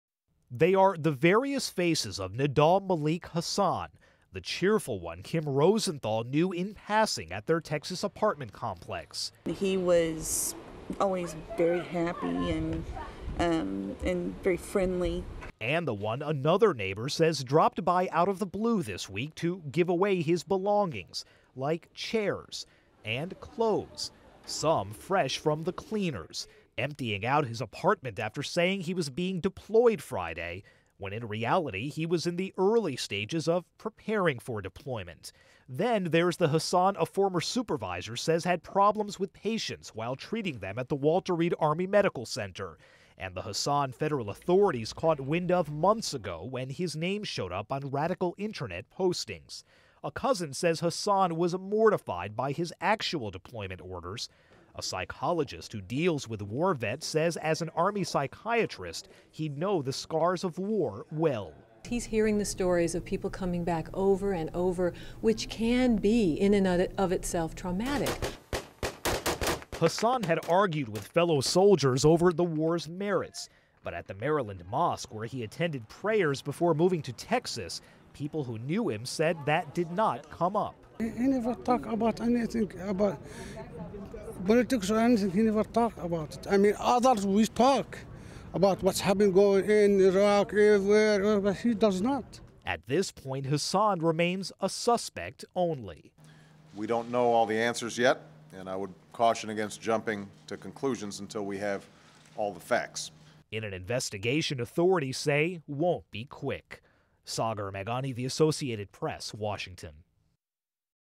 Investigators examined Maj. Nidal Malik Hasan's computer, his home and garbage Friday to learn what
(6 Nov 2009) HEADLINE: Fort Hood suspect said goodbyes before attack
CAPTION: Investigators examined Maj. Nidal Malik Hasan's computer, his home and garbage Friday to learn what motivated the suspect, who lay in a coma, shot four times after officials say he went on the killing spree that left 13 people at Fort Hood dead. (Nov. 6)
 [Notes:ANCHOR VOICE]
(Hasan headshot)
they are the various faces of Nidal Malik Hasan
(Rosenthal cutaway or apartment complex scene  TX MATTHEWS APARTMENT)
the cheerful one Kim Rosenthal and other neighbors at his Texas apartment complex knew in passing
SOT: Kim Rosenthal, neighbor    TX MATTHEWS APARTMENT  13:19:02
"He was always very happy, always very friendly"
(wide shot of tables and chair 13:11:03
and the one another neighbor says dropped by out of the blue this week to give away his belongings
(tight chairs 13:11:17)
like chairs
(pan down clothes 13:14:24)
and clothes
(wide dry cleaners bag 13:15:20  AND  tight bag tag with "Hasan" 13:15:28
some fresh from the cleaners
(the graphic we talked about -- headshot superimposed over map showing Iraq and Afghanistan)
cleaning out his apartment after saying he was being deployed Friday..when in reality, he was in the early stages of PREPARING for deployment
(FILE Walter Reed exterior or patients inside)
then there's the Hasan a former supervisor says had problems with patients while treating them at the Walter Reed Army Medical Center
(FILE any good computer shots -- people typing, or something that depicts cyberspace)
the one federal authorities caught wind of months ago when his name showed up on radical Internet postings.
(New York Times logo with lettering "He was mortified by the idea of having to deploy)
A cousin said Hasan was mortified by his ACTUAL deployment orders
(Van Dahlen walking  DC FORT HOOD SHRINK INTV 16:27:57)
a psychologist who deals with war vets says as an Army psychiatrist, he'd know the scars of war well
SOT: Barbara Van Dahlen, psychologist  DC FORT HOOD SHRINK INTV  16:23:07
"He's hearing the stories of people coming back over and over, which can be in and of itself traumatic."
(NAT UP FILE any good Iraq or Afghanistan shooting/violence)
Hasan had argued with fellow soldiers over the wars' merits
(mosque exterior sign  MD MUSLIM CENTER 15:30:37, AND closeup of top 15:02:30
but at the Maryland mosque where he attended prayers before moving to Texas
(people walking out 15:03:55)
people who knew him said that didn't come up
SOT: Hani Hasnes, Muslim Community Center member  MD MUSLIM CENTER  15:59:48
He never talk about anything about politics or anything, he never talk about.  Others would talk about what's happening in Iraq or everywhere, he does not."
(aftermath from the Pentagon video last night)
at this point, Hasan remains a SUSPECT only
SOT: President Barack Obama
"We don't know all the answers yet. And I would caution against jumping to conclusions until we have all the facts."
(Any good Fort Hood video today -- soldiers hugging, etc)
in an investigation authorities say won't be quick
SM, The Associated Press, Washington